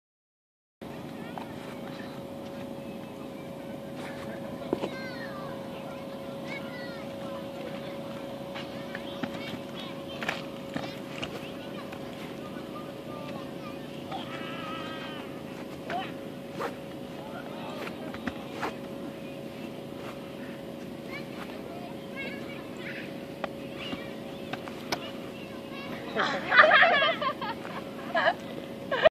20131130 Stilt Ninja action
Just a funny clip from stilt practice